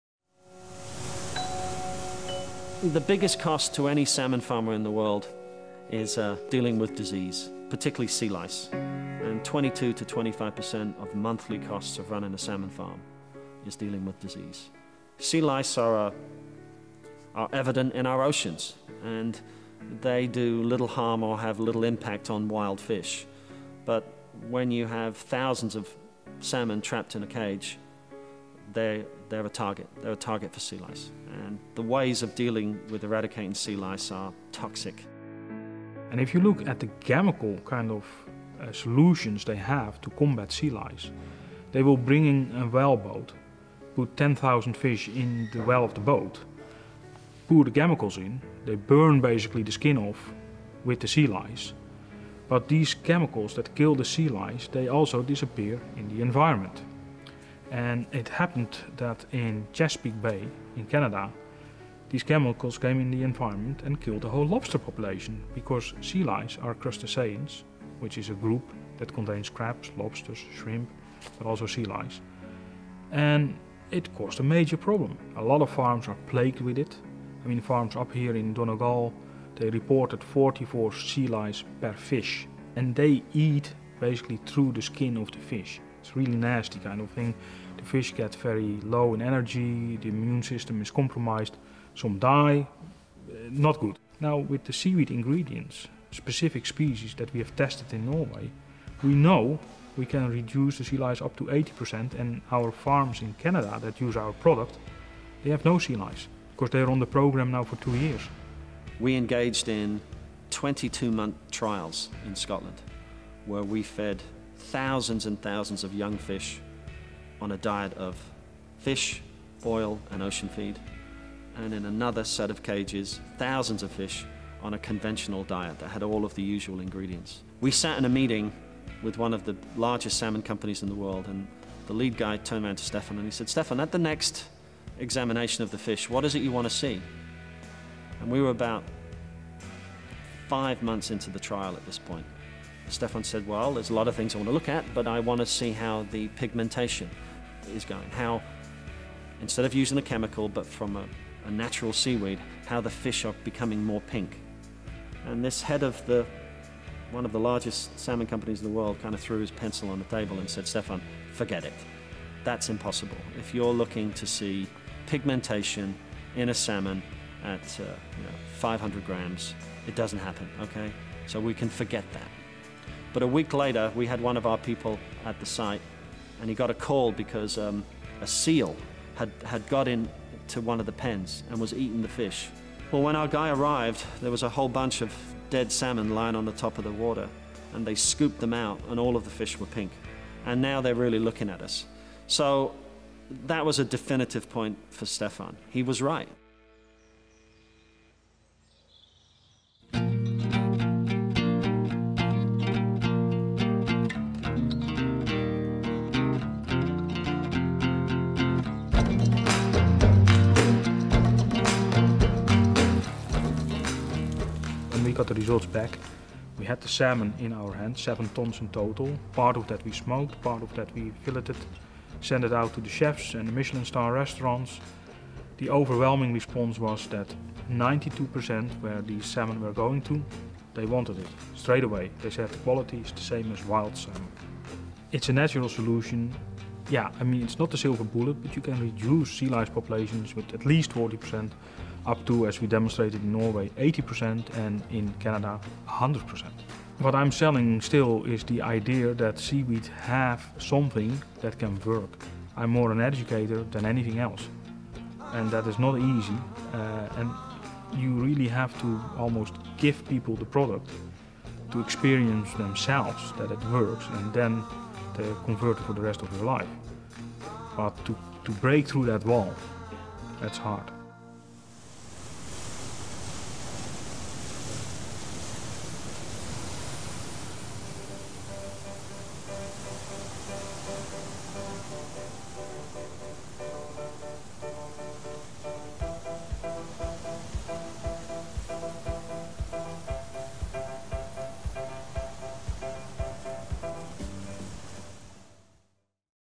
OceanFeed Salmon
Dr.Stefan Kraan and Patrick Martin talk about the issues the salmon industry faces today and their journey to developing OceanFeed Salmon.